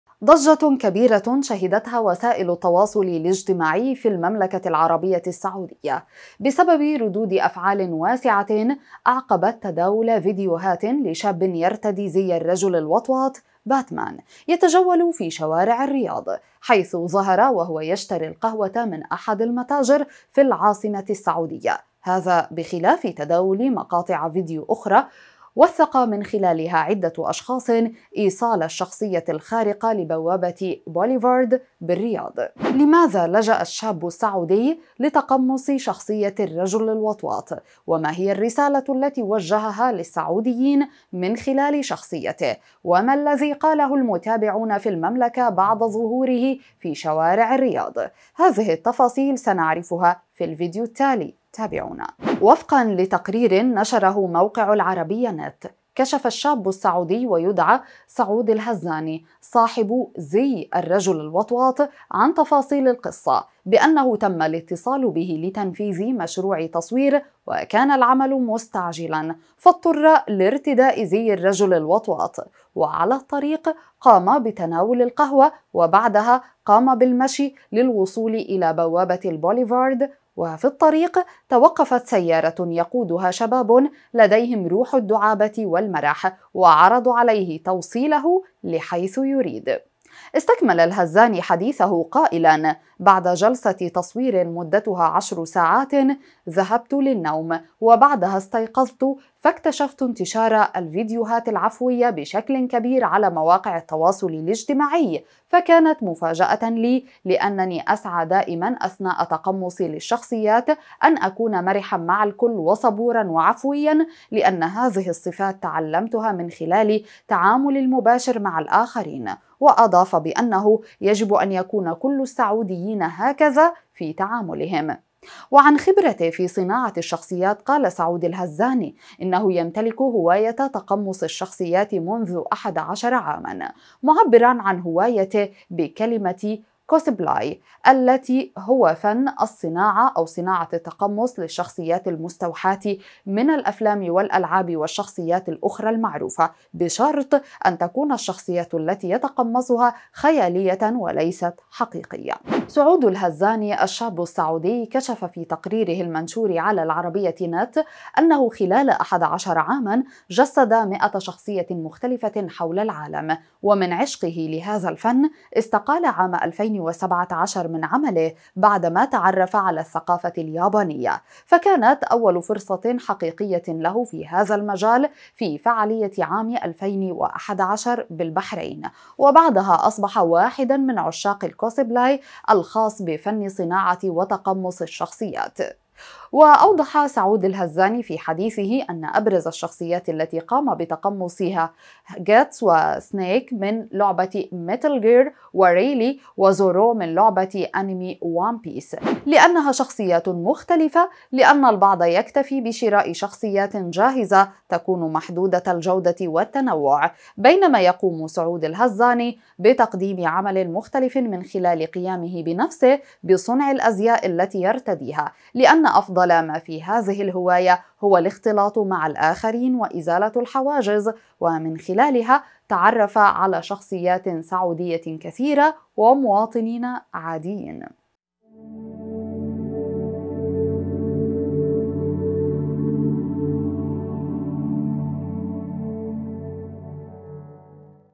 الرجل الوطواط يثير ضجة في شوارع الرياض.. لماذا تقمص شاب سعودي شخصية باتمان ولهذا السبب ترك عمله!
الرجل الوطواط يثير ضجة في شوارع الرياض.. لماذا تقمص شاب سعودي شخصية باتمان ولهذا السبب ترك عمله ليجسد 100 شخصية في العالم!